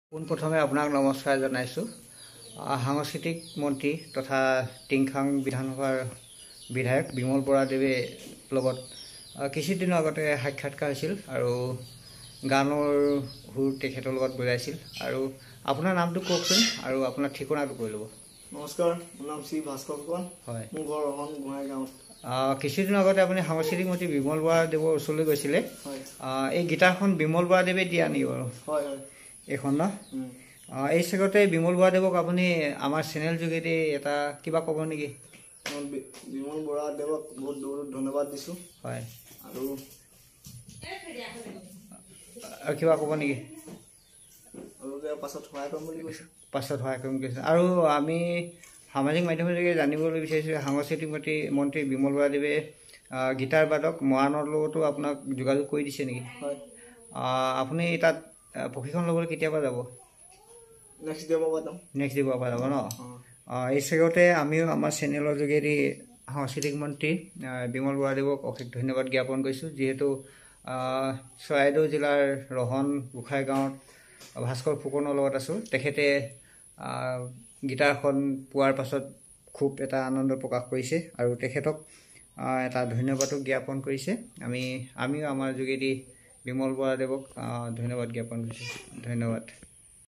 বিমল বৰা,সাংস্কৃতিক মন্ত্ৰীক ধন্যবাদ, ভাস্কৰ ফুকনে গিতাৰ উপহাৰ পোৱাৰ বাবে bmtsk Tourism project
চৰাইদেউ জিলাৰ ৰহন ,গোহাঁই গাঁওৰ ভাস্কৰ ফুকনে  অসম চৰকাৰ উদ্যোগ, বাণিজ্য আৰু ৰাজহুৱা প্ৰতিষ্ঠান তথা সাংস্কৃতিক পৰিক্ৰমা বিভাগৰ মন্ত্ৰী

সন্মানীয় বিমল বৰা দেৱৰ পৰা এখন গীটাৰৰ উপহাৰ স্বৰূপে পোৱাৰ বাবে অশেষ ধন্যবাদ জনাই।

এই চেগতে আমিও মাননীয় মন্ত্ৰী মহোদয়ক অশেষ ধন্যবাদ জনাইছোঁ।

কিছু দিনৰ আগতে বিমল বৰা দেৱ ক সাহায্য বিচাৰি ভাস্কৰ ফুকনে মন্ত্ৰীৰ কাষ চাপিছিল আৰু মন্ত্ৰী দেৱে অনুৰোধ কৰিছিল ভাস্কৰ ফুকনক গীত পৰিৱেশনৰ বাবে আৰু গীত পৰিৱেশন কৰাৰ লগতে  আৰু সুৰ মিলাই ছীল মন্ত্ৰী মহোদয়ে।

সামাজিক মাধ্যমত ও বহল ভাৱে প্ৰচলিত হৈছিল ভাস্কৰ ফুকনৰ গীতৰ লগতে মন্ত্ৰী গীত পৰিৱেশন ৰ।

মন্ত্ৰী মহোদয়ে গান শুনাৰ পাছত নিজে ভাস্কৰ ফুকনৰ ঘৰত গৈ এখন গীটাৰ আৰু মৰাণ ৰ গীটাৰ বাদক উৎপল গগৈ ওচৰত প্ৰশিক্ষণৰ বাবে ব্যৱস্থা কৰি দিয়ে।

 মন্ত্ৰী মহোদয়ে নিজে ঘৰত গৈ গীটাৰ দি অহাৰ বাবে ধন্যবাদ জনাই ভাস্কৰ ফুকনে আৰু ঘৰৰ পৰিয়াল বৰ্গ ৰ তৰফৰ পৰা আদৰণি জনায় আৰু ধন্যবাদ জ্ঞাপন কৰে।

আগলৈ ক্ৰমে। ভাস্কৰ ফুকনৰ গীতৰ শৰাই।